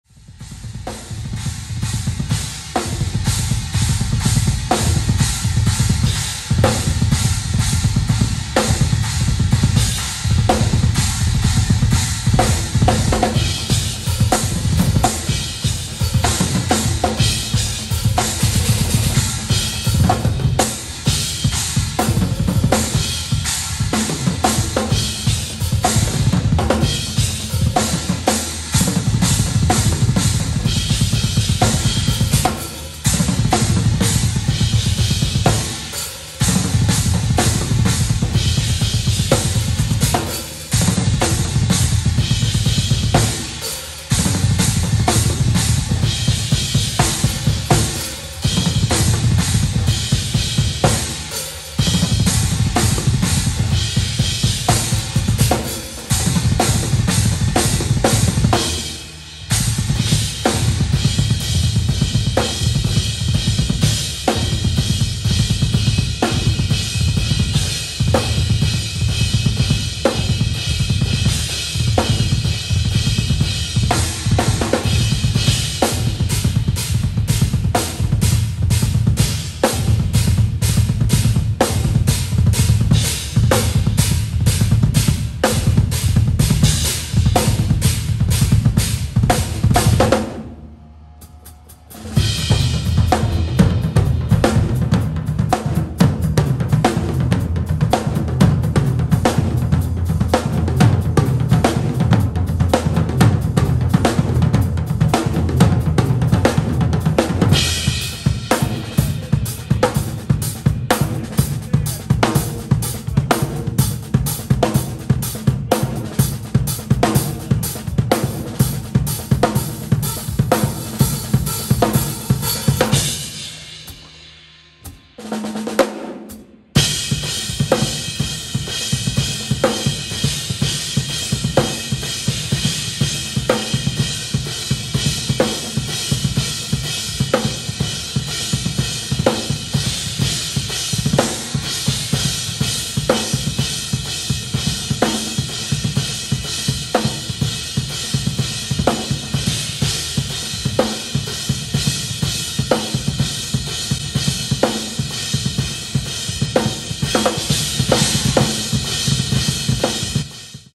Matt Halpern's Pasic 21 Soundcheck- Periphery
Matt Halpern 11-12-21 Pasic 21 Soundcheck. Matt is best known for his work with progressive metal band Periphery. Drummer Photographer.